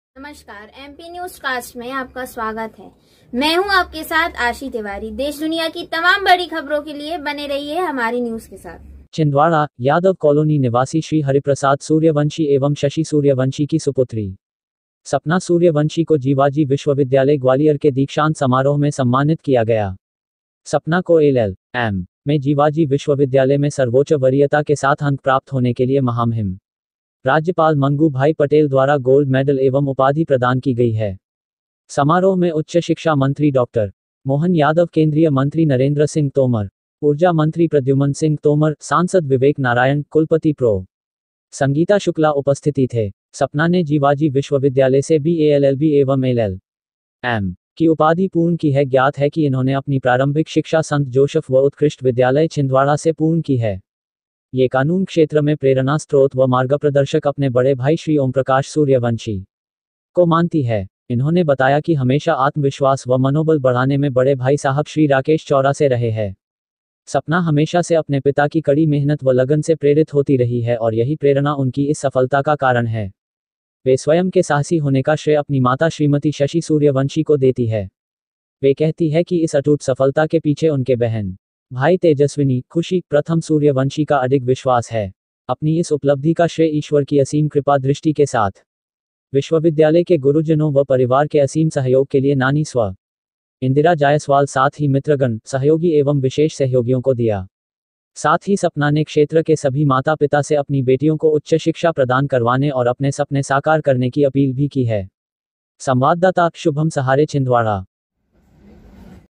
किसान पुत्री सपना को मिला कानून स्नातकोत्तर में गोल्ड मेडल MPNEWSCAST
चैनल को सब्जक्राइब करिऐ नोटिफिकेशन चालू करिऐ